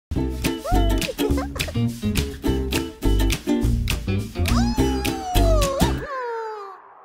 BALLOONS
Harbinger Media partnered with Orchid Creation to produce a series of shorts combining live action and animation featuring Benefit's cosmetics Lil' Miss Happy.